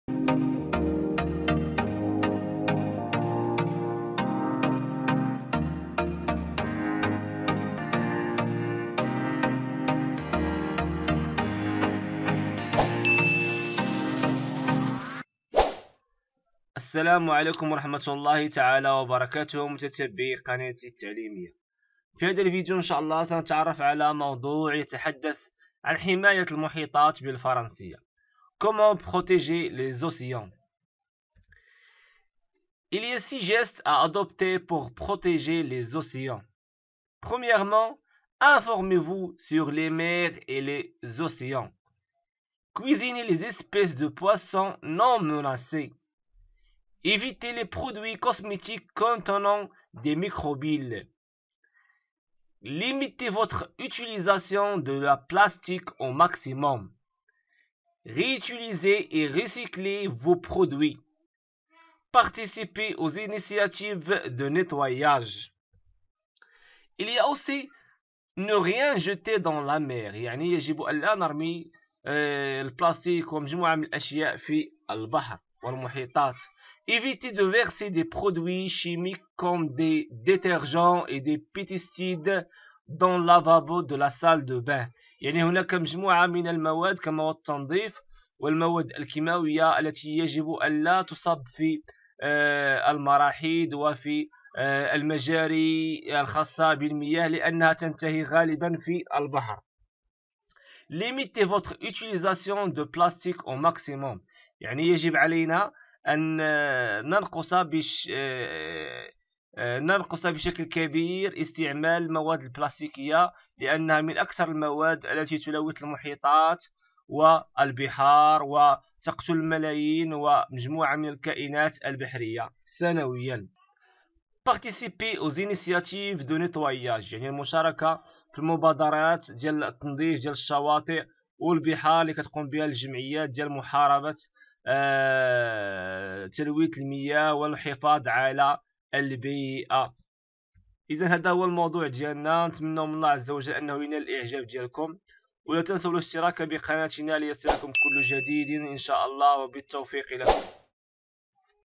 comment protéger les océans موضوع يتحدث عن حماية المحيطات بالفرنسية
comment protéger les océans
موضوع يتحدث عن حماية المحيطات بالفرنسية